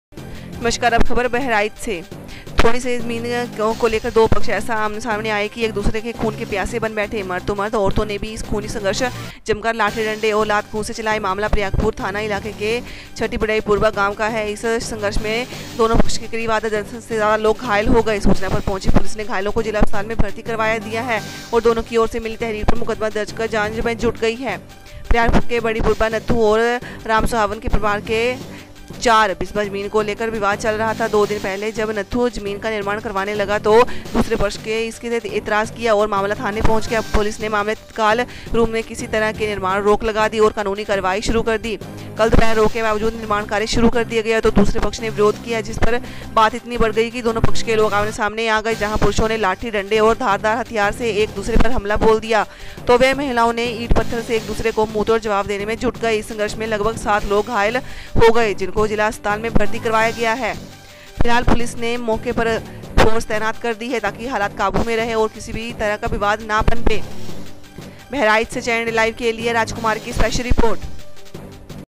ज़मीनी विवाद में खुनी संघर्ष.6 घायल Channel India Live
बहराइच से राज कुमार की रिपोर्ट